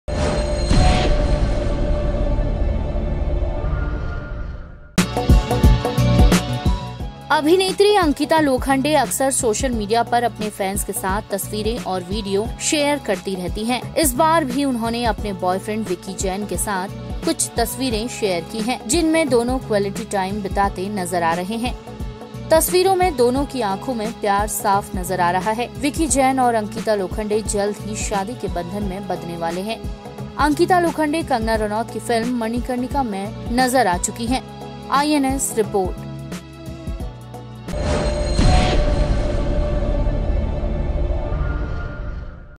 Ankita Lokhande ने बॅायफ्रेंड Vicky Jain के साथ शेयर की Romantic picture
अभिनेत्री अंकिता लोखंडे अक्सर सोशल मीडिया पर अपने फैंस के साथ तस्वीरें और वीडियो शेयर करती रहती हैं , इस बार भी उन्होने अपने बॉयफ्रेंड विक्की जैन के साथ कुछ तस्वीरें शेयर की है ।